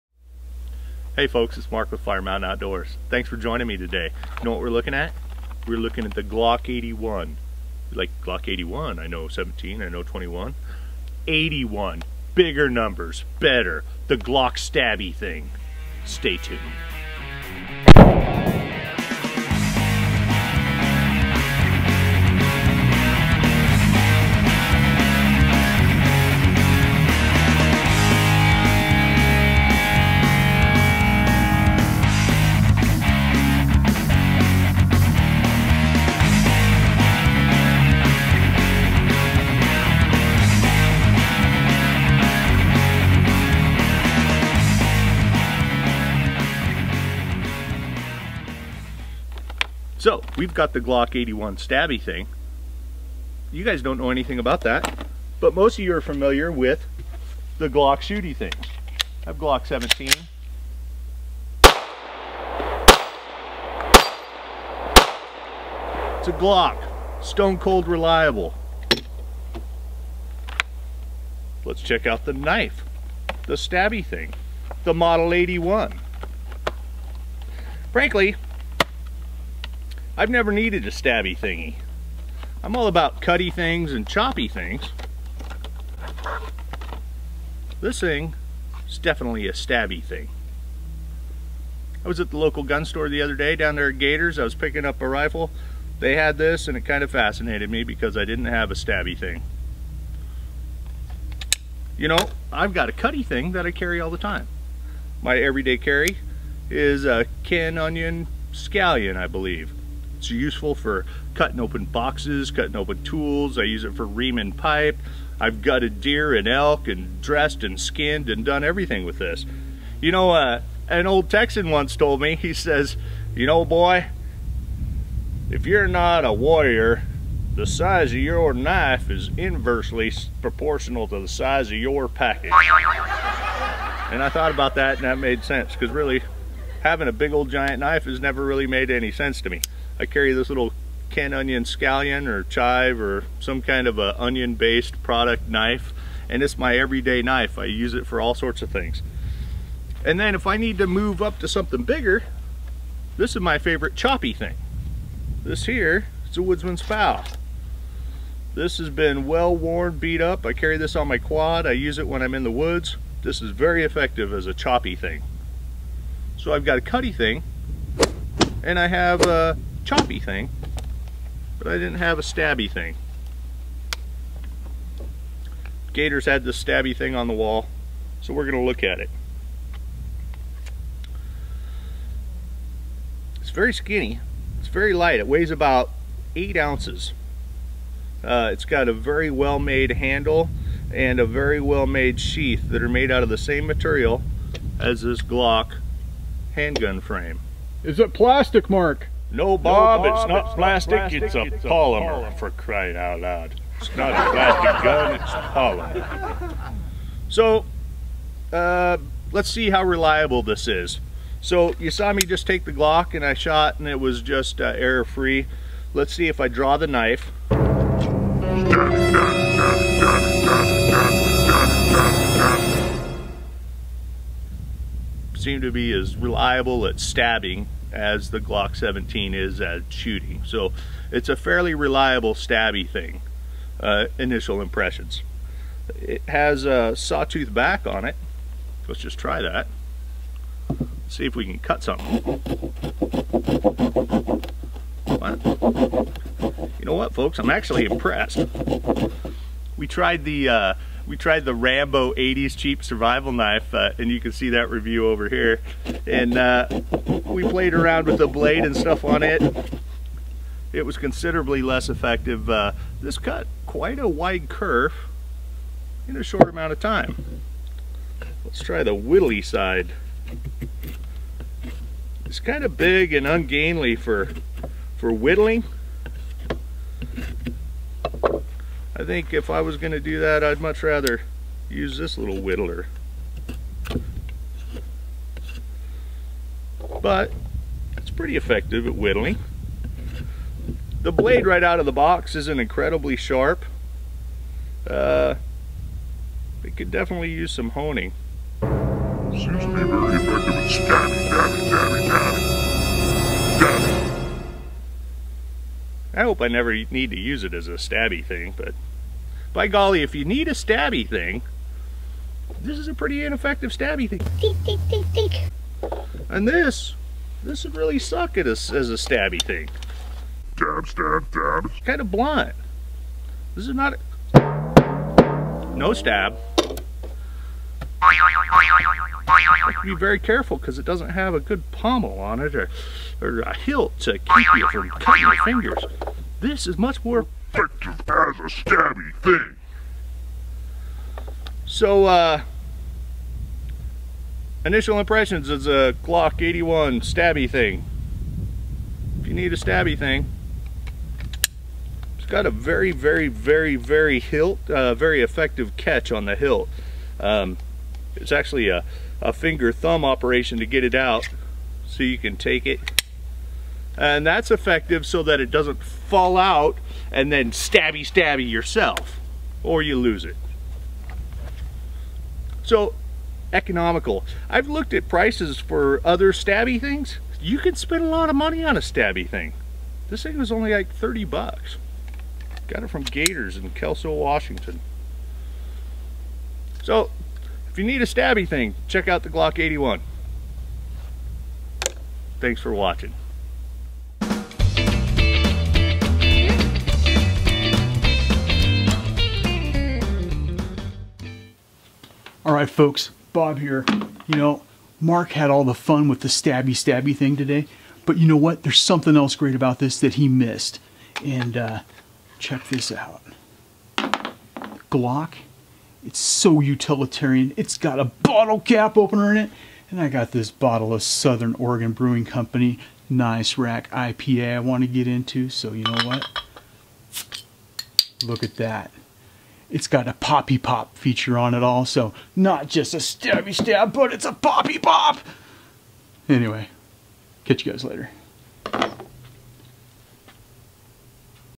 Glock Stabby Thing!!! Glock 81 Review and Evaluation!!
Follow Mark from Fire Mountain Outdoors as he tests and evaluates the Glock 81 Survival Knife with Saw. Is it just as reliable as a Glock G17 9mm shooty thing? Find out as we test the pros and cons of this polymer handled survival knife.

Integrated saw
Total weight 7.13oz.
Blade is phosphate treated spring steel.

The G81 saw-back survival knife is a perfect addition to any tool kit, field pack or belt. Developed in close cooperation with Austrian Army Rangers, the no-nonsense GLOCK knife features a tough 6 1/2 inch carbon steel clip point blade and molded polymer hilt, tough enough to break windows or cut through the toughest tree limb, yet perfectly balanced for appropriate conditions. The one-piece polymer sheath is ideal for keeping the 3/16 inch blade protected. The set includes an integral retention system to keep your GLOCK knife secure during times of extreme activity. Available in olive drab, desert sand, or midnight black.

Also used in this video are the Ken Onion Kershaw Knife and a vintage US Military LC-14-B Woodsman's Pal.

The LC-14-B Woodman's Pal survival axe was originally a tool for forestry or agriculture, but quickly earned a place as an outstanding military implement. The well designed and balanced blade was first procured in World War II. The LC-14-B Woodman's Pal served through the Vietnam War and is still part of the military inventory of edged tools in the 21st Century.

Also... it has as bottle opener. So watch as Bob sneaks a bottle of Southern Oregon Brewing Companies Nice Rack IPA into the testing facility to try out that feature as well.